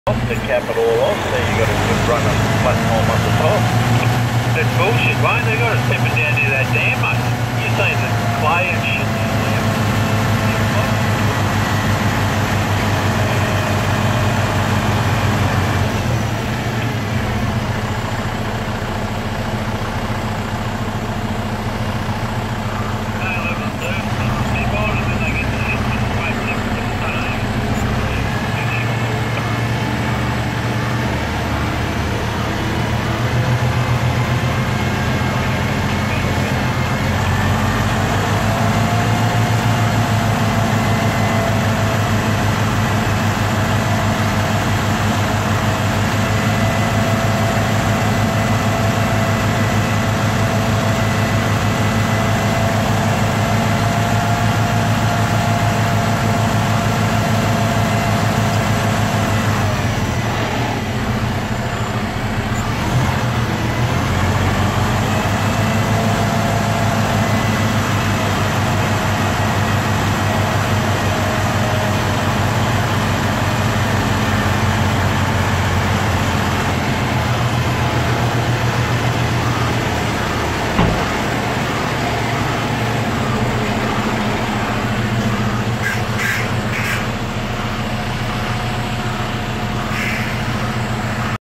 Reversing 5 axle dog trailer truck and dog
Truck and dog freightliner coronado..quin dog pbs 63ton